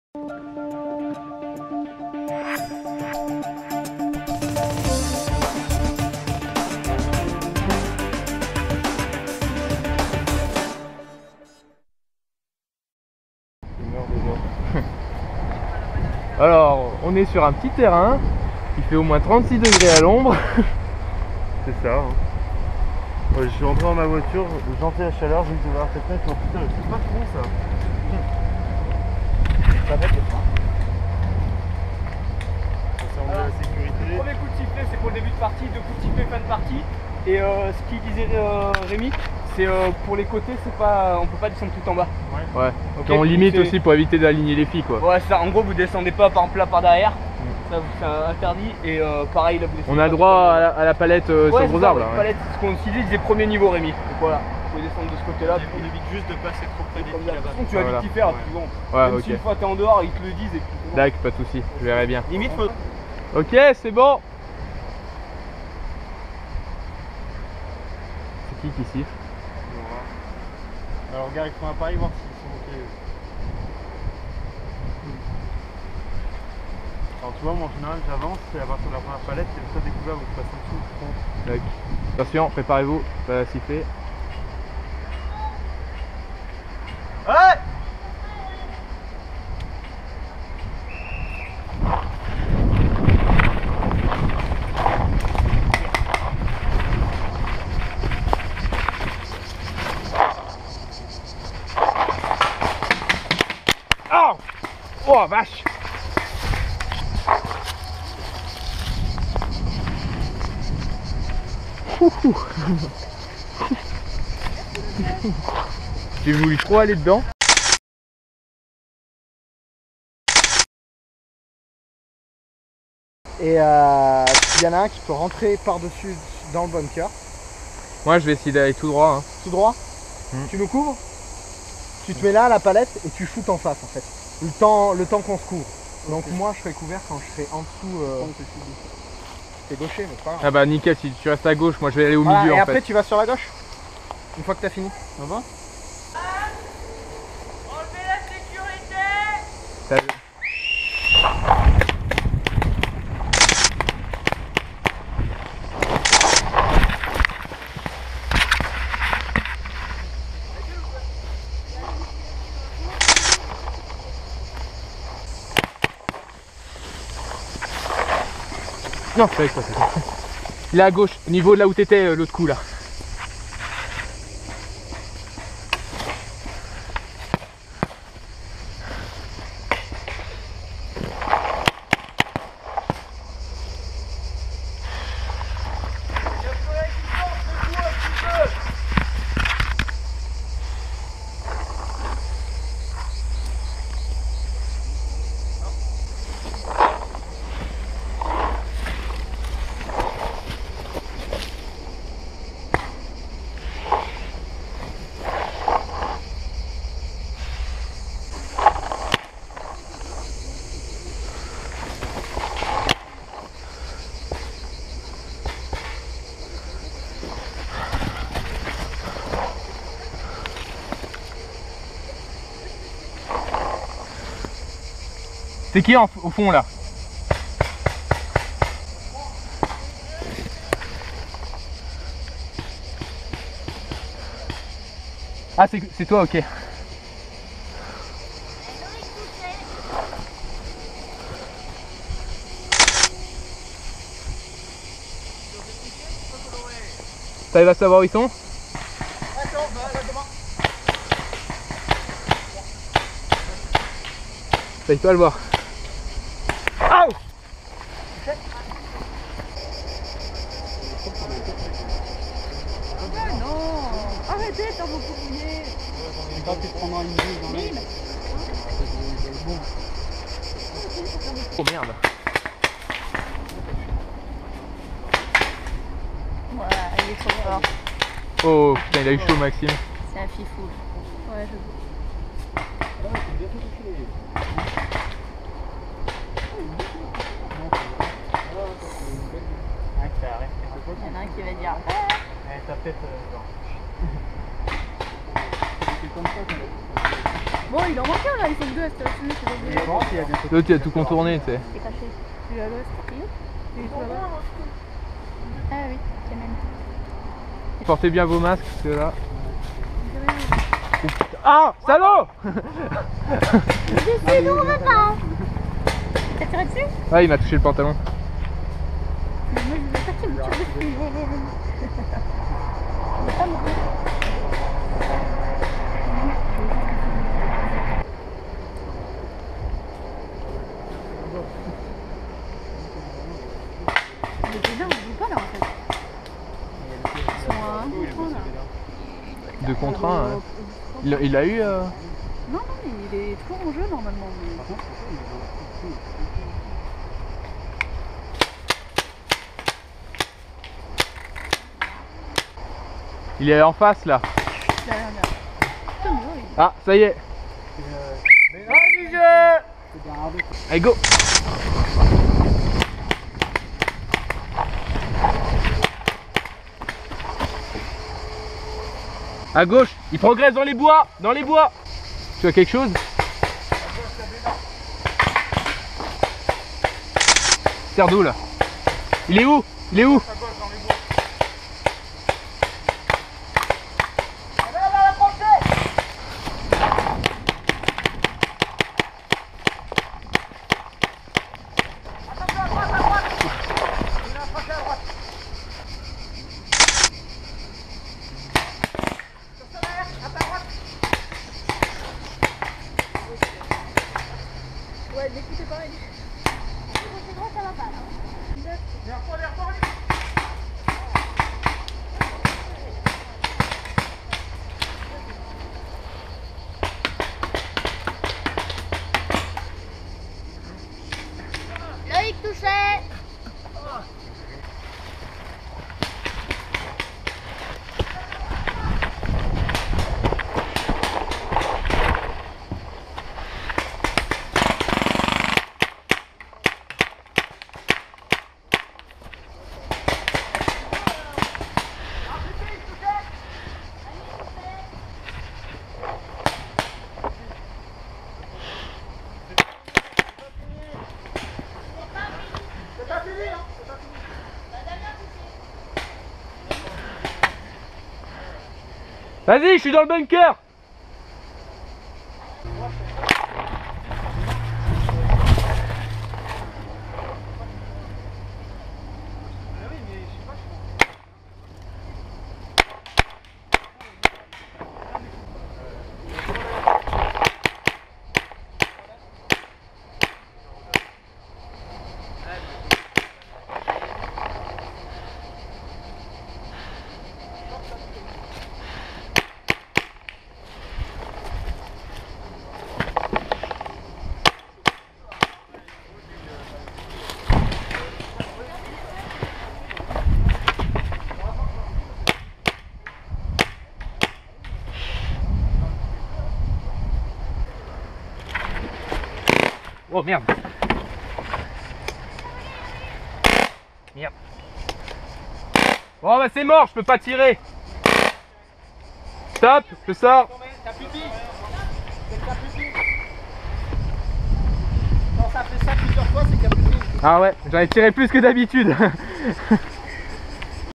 Paintball gameplay
Caméra : Tête gopro hero 3+ black edition
Scopecam : mobius modifiée

english
Hi everyone

Caméra : head gopro hero 3+ black edition
Scopecam : mobius modified